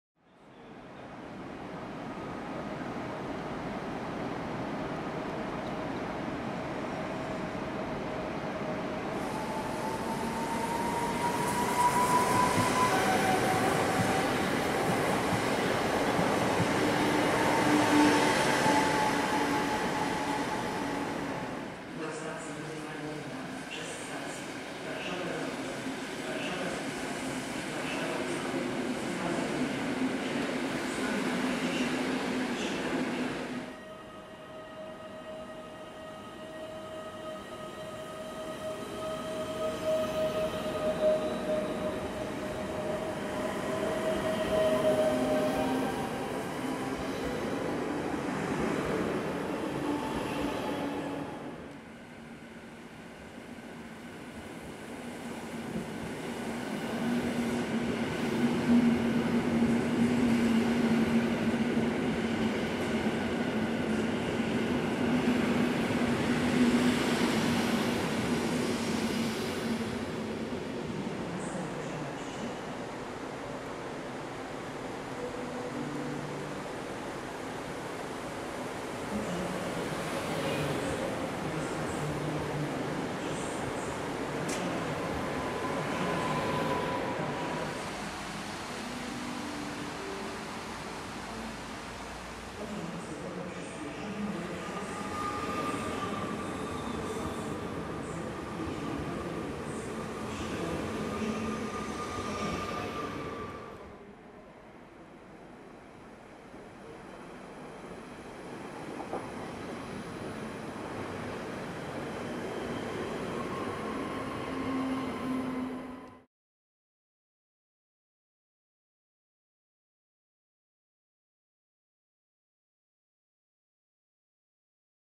Pociągi regionalne na dworcu Kraków Główny
Kilka składów regio nagranych na stacji Kraków Główny.

Spis treści:
"00:00 EN77-004 jako pociąg KMŁ na lotnisko
"00:21 EN71-001 jako Regio Nikifor z Krynicy Zdroju
"00:33 EN99 w duecie z EN64 jako Regio do Katowic
"00:51 Duet EN57FPS jako Regio z Katowic
"01:11 EN78-009 jako Regio z Katowic
"01:28 22WEd-011 jako Gibki Cug z Katowic
"01:44 EN64-001 jako Regio z Rzeszowa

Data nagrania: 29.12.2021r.
Kamera: SONY HDR-CX405
Miejsce: Kraków Gł.